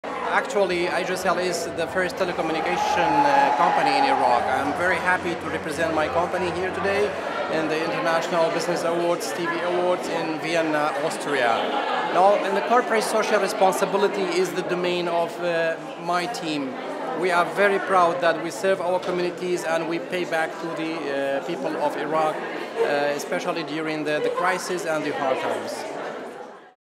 Asiacell wins a Stevie® Award in The 2019 International Business Awards
Winners in The 2019 International Business Awards were celebrated and presented with Stevie® Awards in Vienna, Austria on 19 October.